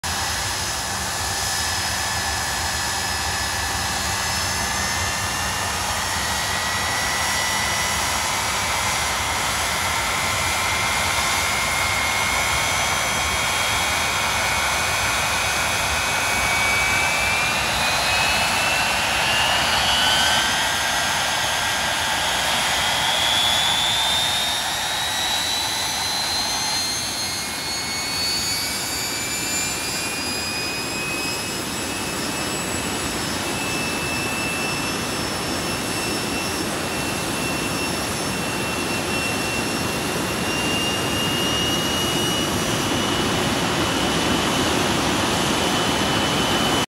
Blackburn Buccaneer high power engine run
Blackburn Buccaneer XW544 conducting a high power engine run at the TBAG and Threshold aero organised photoshoot held at Cotswold airport (Kemble) March 2023